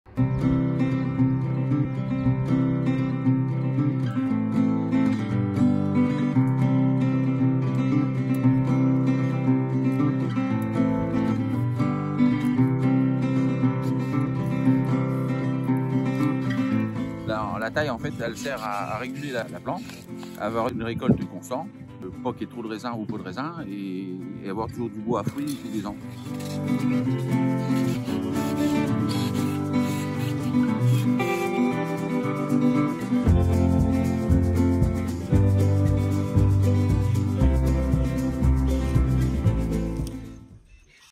La Taille 2023
Nous rejoignons l'équipe dans nos vignes à Landreville pendant la période de la taille.

Retrouvez nous également sur Instagram, Facebook et LinkedIn !